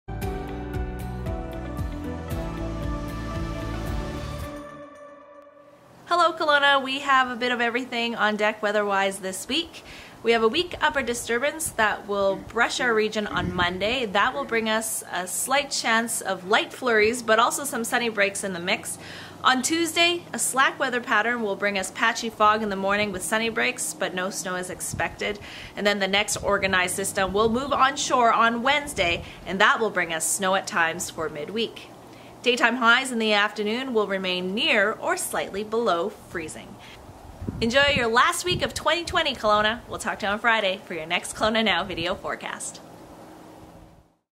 Weather with Wesla: A bit of everything this week
We will see sunny breaks on Monday along with light flurries when a weak upper disturbance brushes the region.

On Tuesday, a slack weather pattern will bring us patchy fog in the morning with sunny breaks.

The next organized system will move onshore on Wednesday, bringing us snow at times.

Daytime highs will remain near or slightly below freezing.

Have a great final week of 2020, Kelowna!